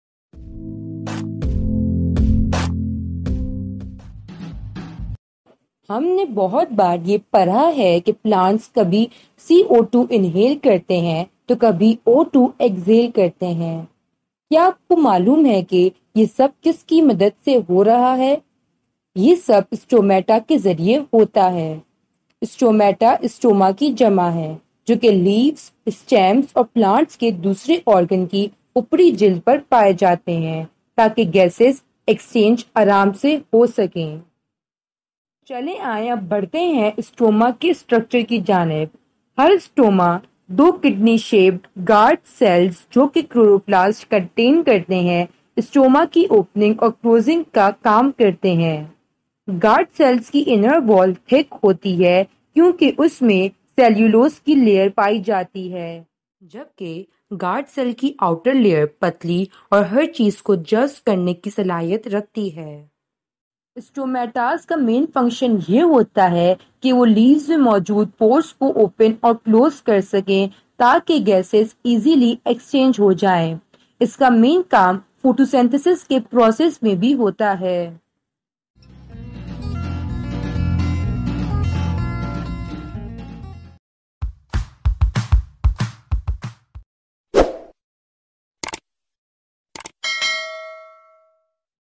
What is stomata | Structure and function of stomata ( Stomatal transpiration )
Hello learners!
In this video, you will learn about stomata. where is stomata found, guard cells, opening and closing of stomata, and transpiration in plants through stomata. We hope that you will find this video informative and helpful.